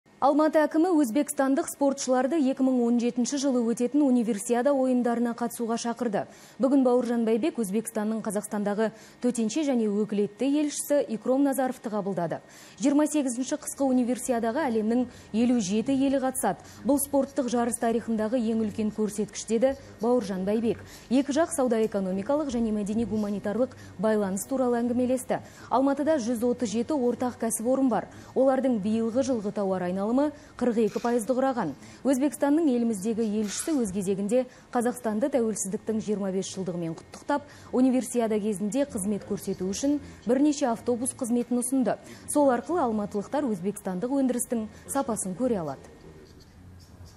Алматы әкімі Өзбекстандық спортшыларды Универсиадаға қатысуға шақырды (11.11.16)
Бауыржан Байбек Өзбекстаннның Қазақстандағы төтенше және өкілетті елшісі Икром Назаровты қабылдады.